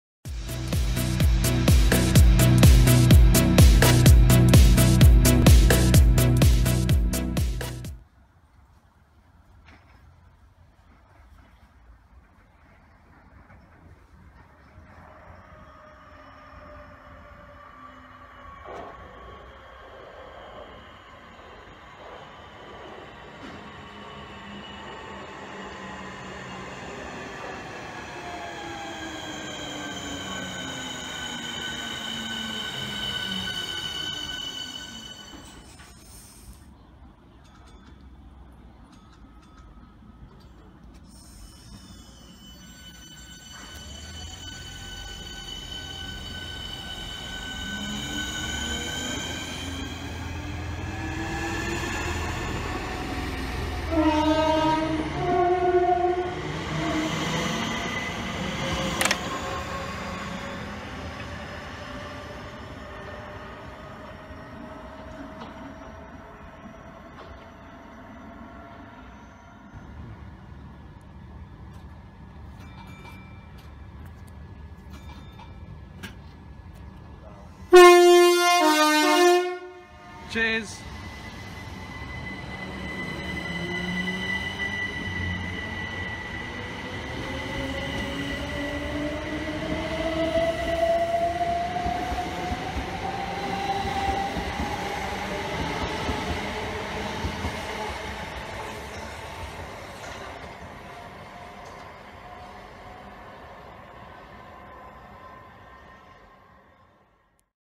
Legend Drivers @ Canterbury East 26/9/16
In todays video we see some legend Southeastern drivers Blasting some amazing tones. First we see 465041 heading to dover priory which is then followed by 375819 and 375309 also heading for dover priory.
Thanks to the drivers for the tones :)